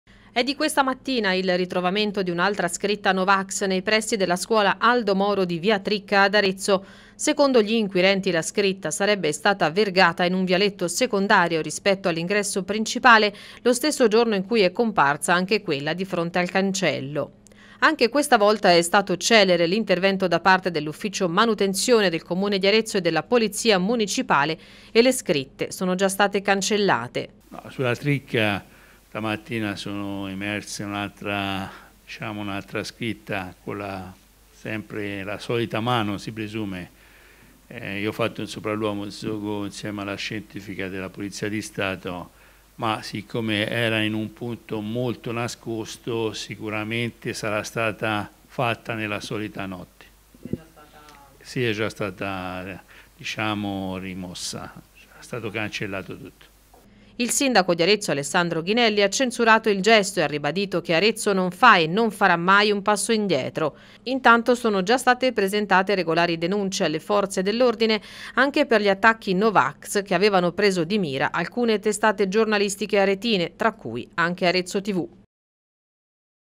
Arezzo, nuove scritte no vax nei pressi della scuola "Aldo Moro"
É di questa mattina il ritrovamento di un' altra scritta no vax nei pressi della scuola "Aldo Moro" di via Tricca ad Arezzo. Secondo gli inquirenti la scritta sarebbe stata vergata in un vialetto secondario rispetto all'ingresso principale lo stesso giorno in cui è comparsa anche quella di fronte al cancello principale.

Anche questa volta è stato celere l'intervento da parte dell'Ufficio manutenzione del Comune e della polizia municipale di Arezzo e le scrtte che ha prontamente rimosso le scritte.

Il sindaco Alessandro Ghinelli ha censurato il gesto ed ha ribadito che Arezzo non fa e non farà mezzo passo indietro. Intanto sono già state presentate regolari denunce alle forze dell'ordine anche per gli attacchi no vax successivi al ritrovamento della prima scritta e che presero di mira alcune testate giornalistiche aretine, tra cui anche Arezzo TV.